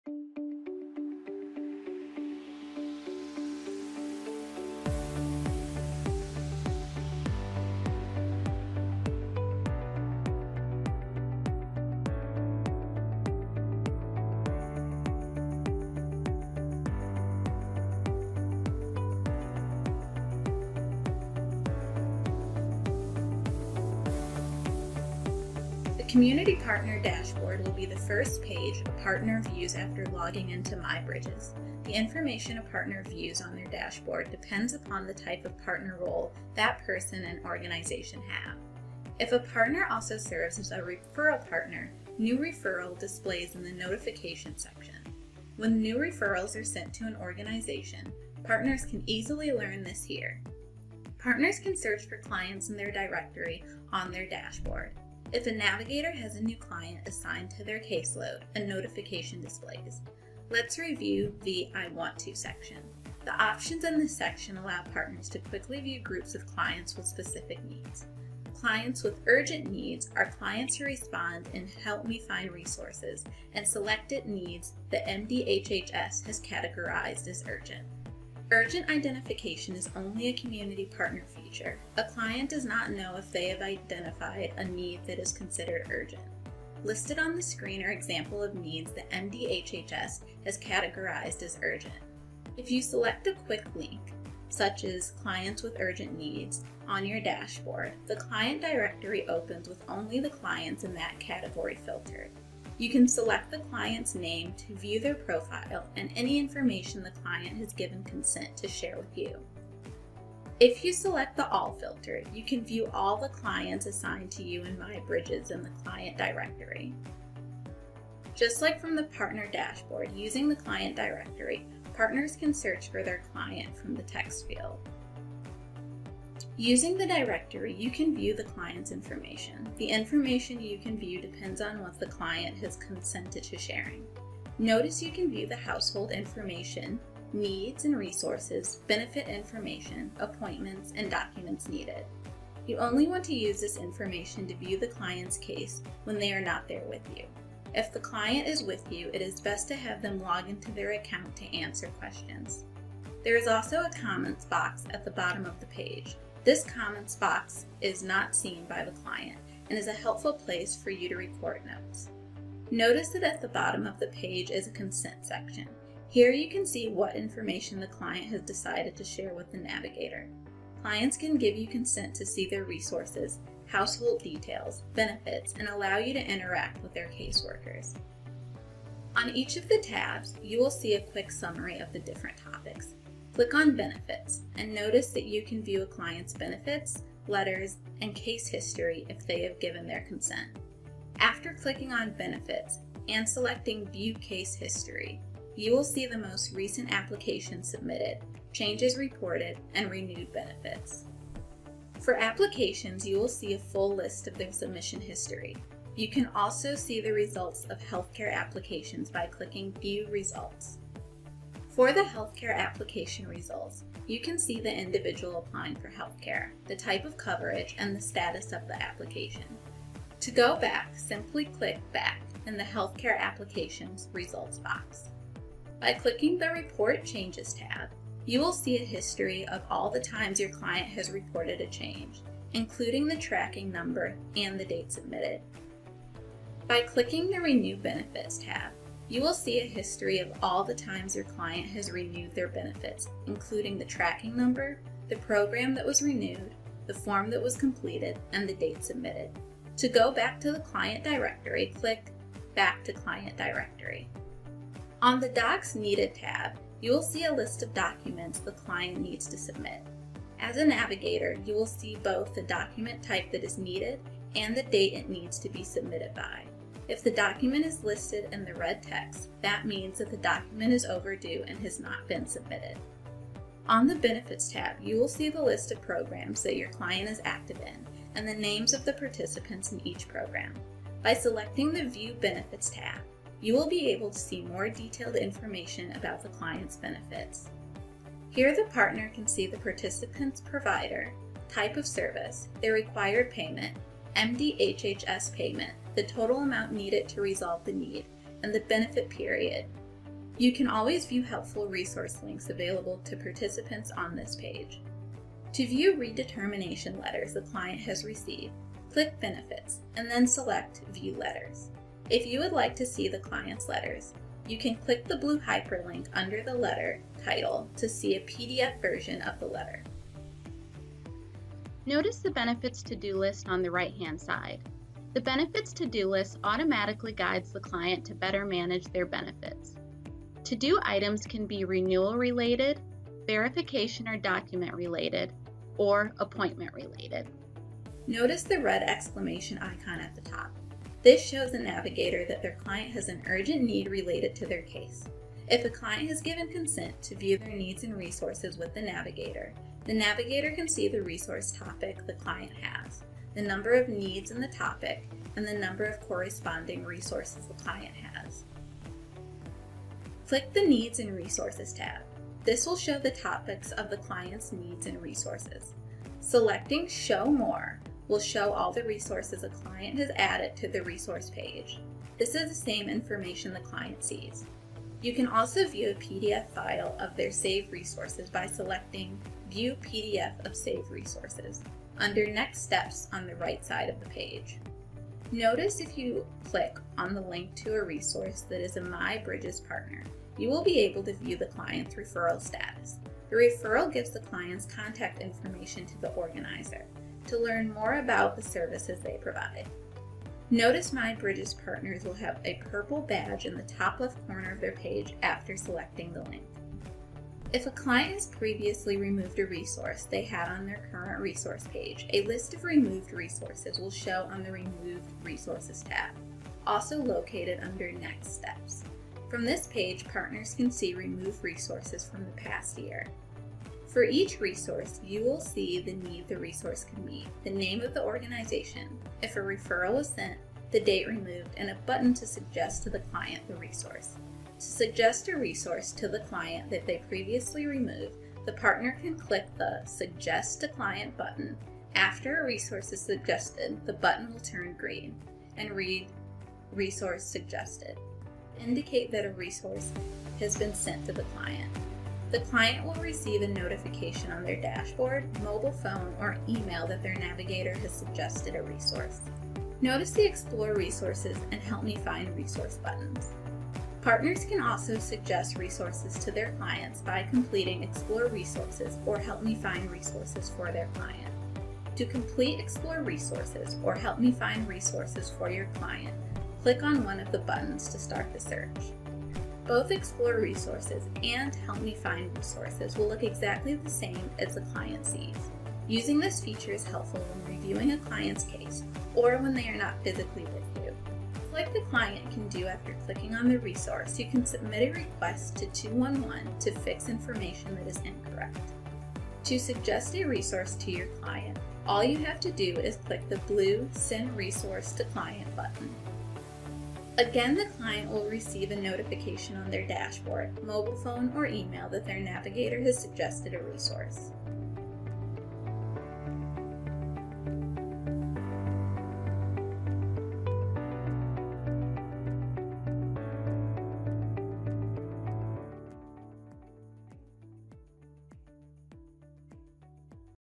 MI Bridges: Manage A Client
Enjoy the “How To” Tutorial for Manage My Case in MI Bridges.

A MUSIC STANDARD LICENSE has been purchased for this video’s audio content.